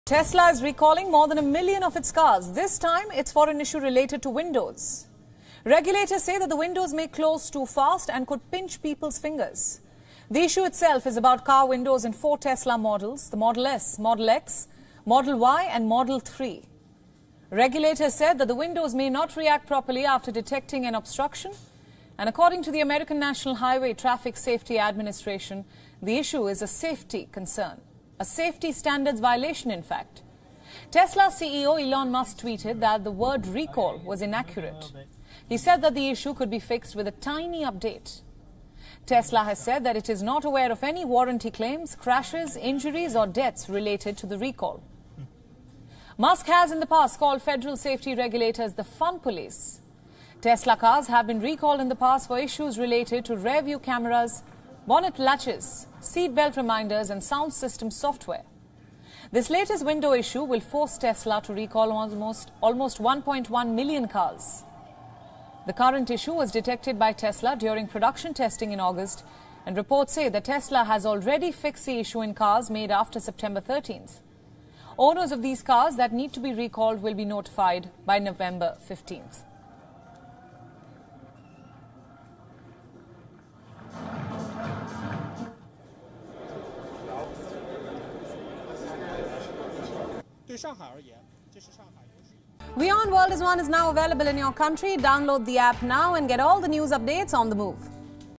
WION Fineprint | Tesla to recall around 1.1 million cars over window issue
U.S. regulators say issue in some Tesla car windows may cause them to 'pinch' people when they roll up. Elon Musk says the issue can be fixed with a 'tiny' software update. Priyanka Sharma gives you the details.